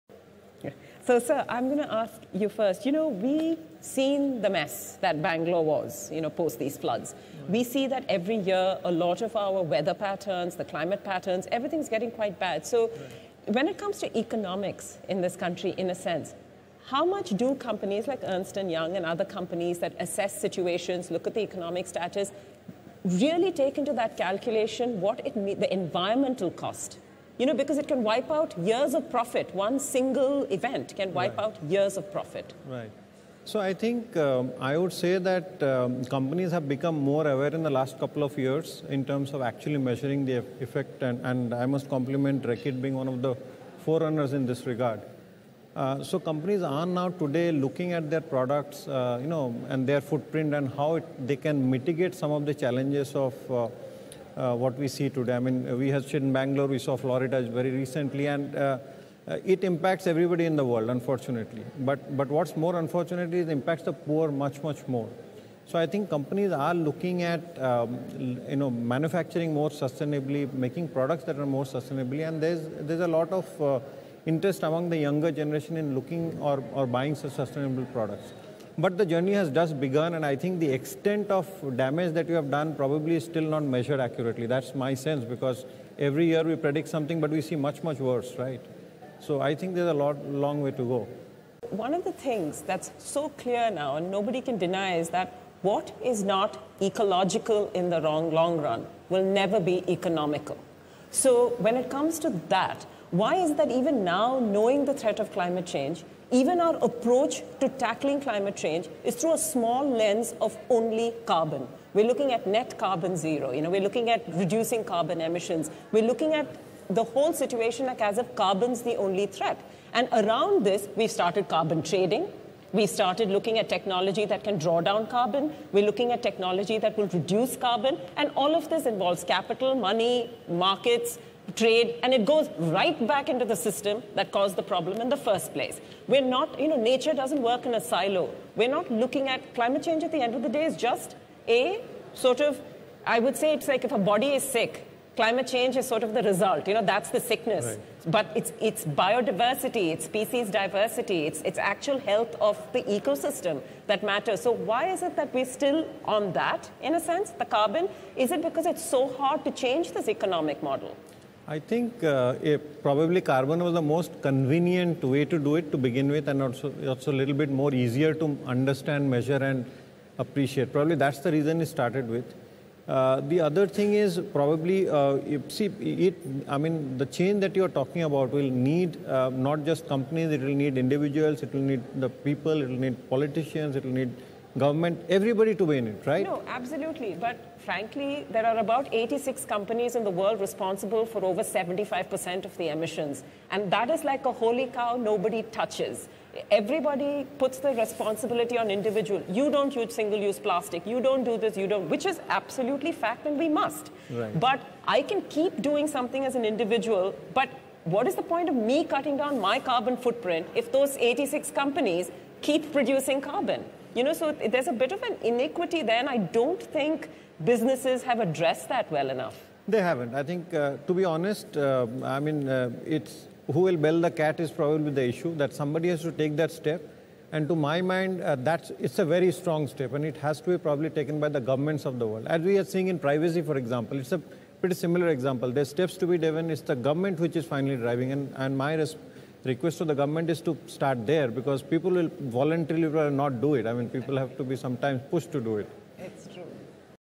Companies Have Become Aware Of The Impact Of Climate Change: Pinakiranjan Mishra
Bengaluru, the IT hub of India, faced one of the worst floods recently. Do companies working towards economic development consider the impact of climate change? At the NDTV-Dettol Banega Swasth India's Lakshya - Sampooran Swasthya Ka telethon, Pinakiranjan Mishra, Consumer Leader, EY India said, "Companies have become aware now of the impact of climate change. It affects the poor more and therefore, more sustainable products are needed and younger generation is taking it up."